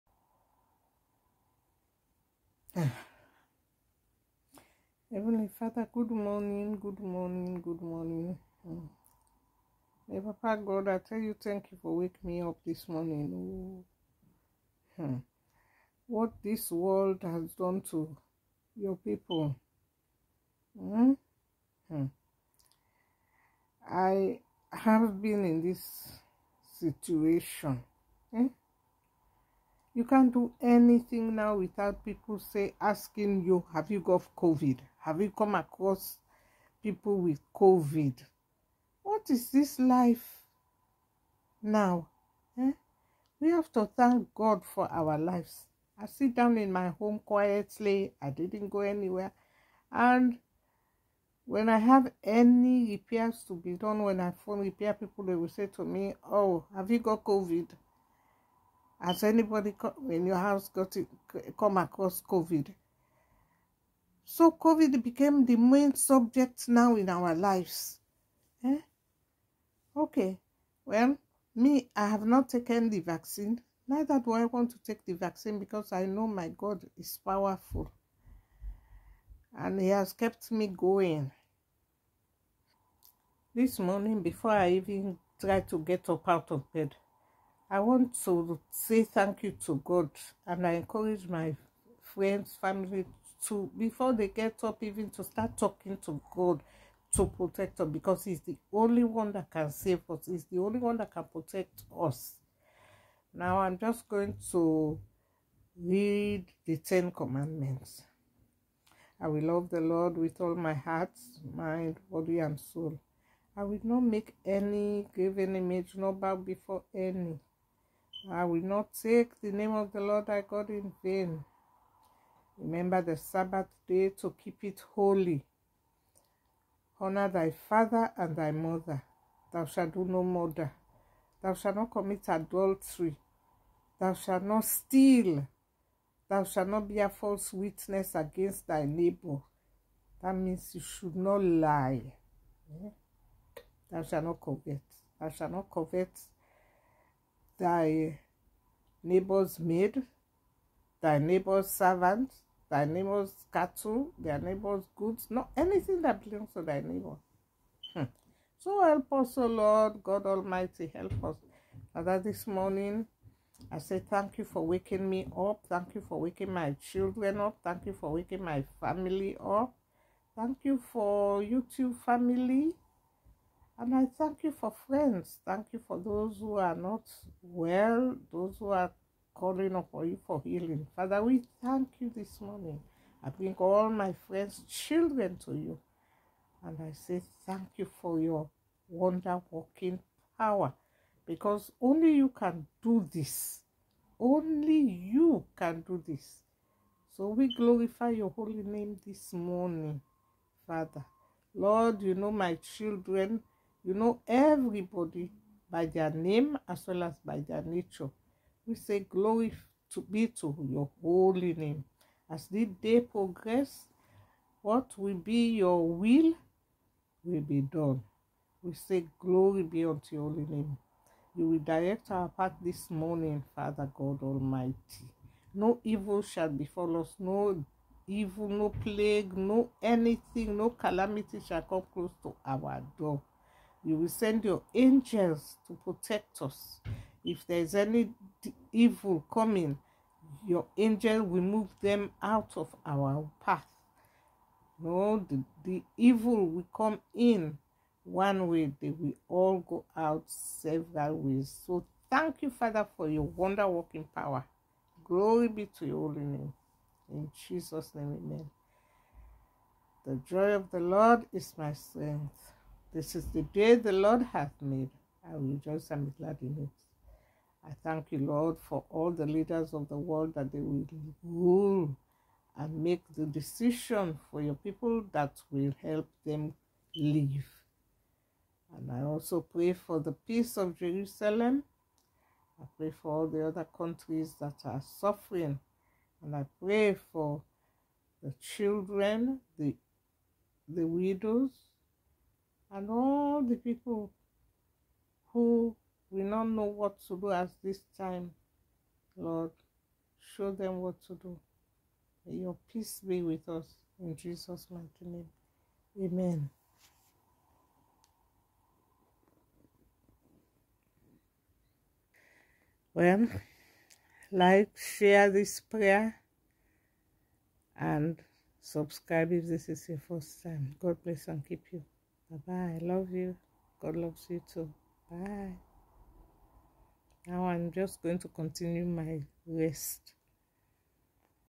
COVID, OMICRON? THE WORKERS ARE NOT COMING.
They are scared. I am cold, the heating has broken down five times. They now ask  have you got Covid-19,  any one in your home? There is no Covid-19 virus in my home. I am praying as I wake up today. I still pray for others. Pray for me. watch the video.